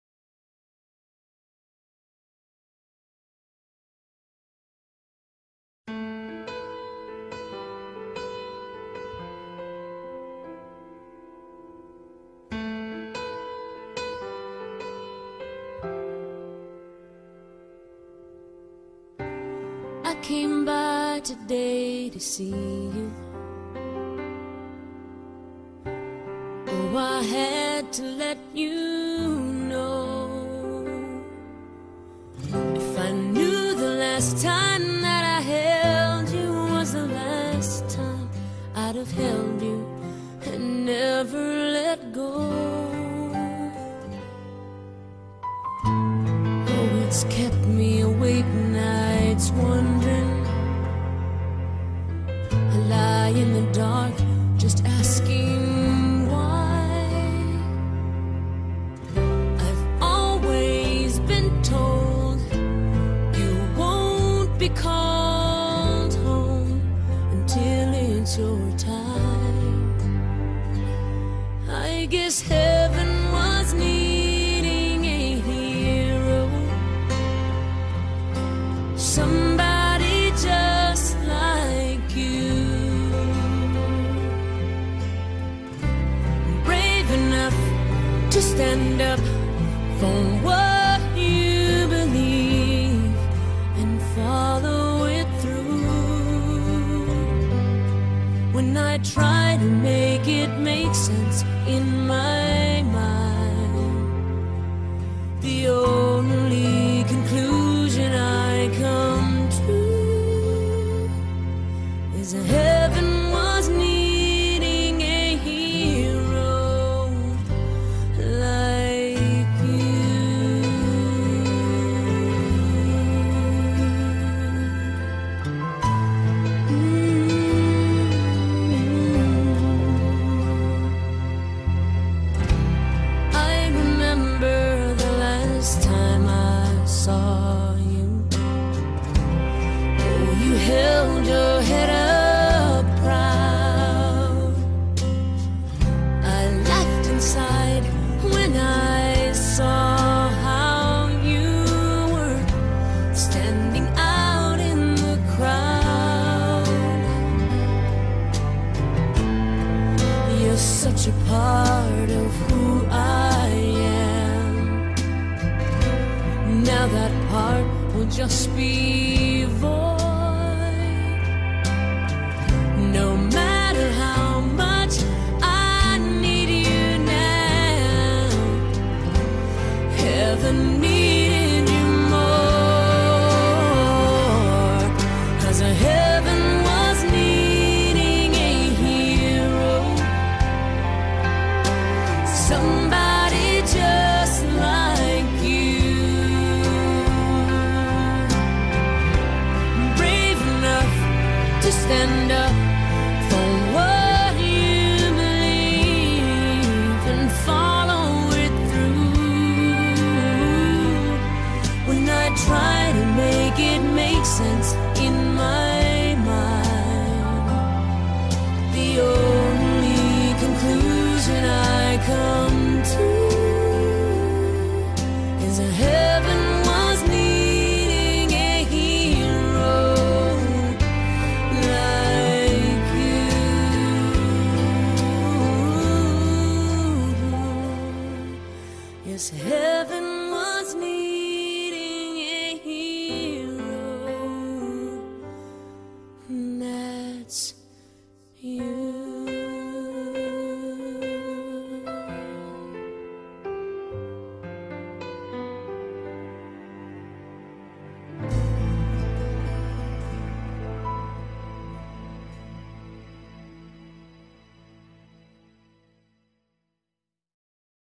Tribute to Dad
We recently lost our father, the greatest man I've ever known. Here's a video I made in his honor.